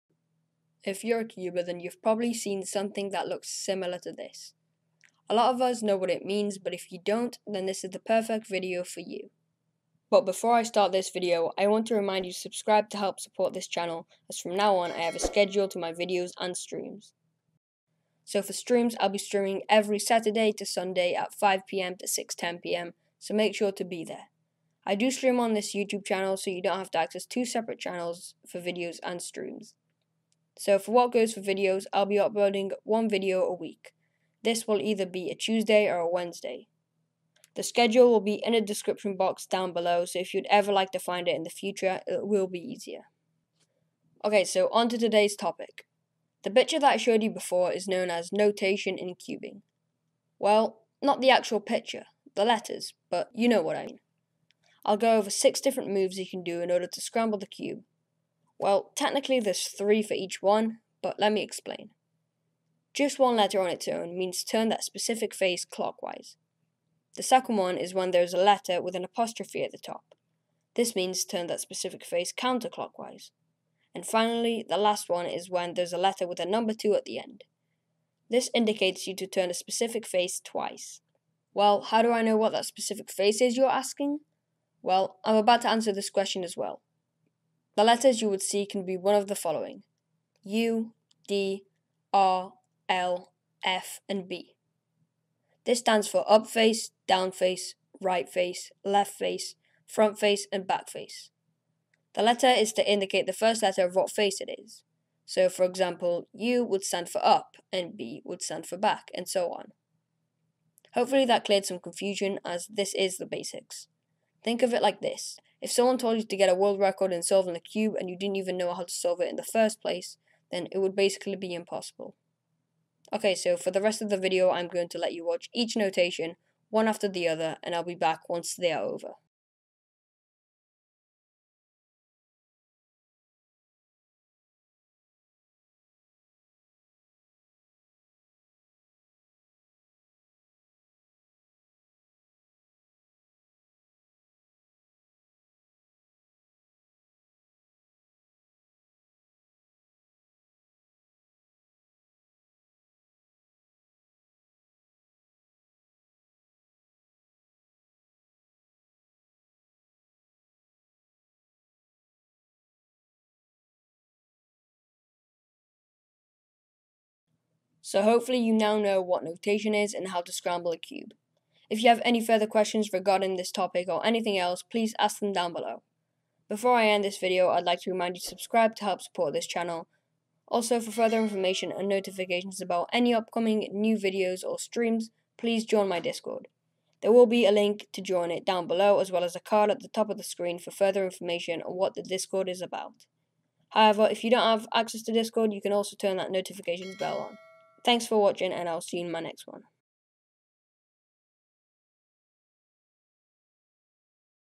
Rubik's Cube: 3x3 Notation - Beginner Tutorial
In this video, I teach how to read notation and how to scramble a 3x3!

ONCE WE HIT 50 SUBS, I WILL HAVE ONE LUCKY PERSON TO JOIN ME IN ONE OF MY STREAMS LIVE!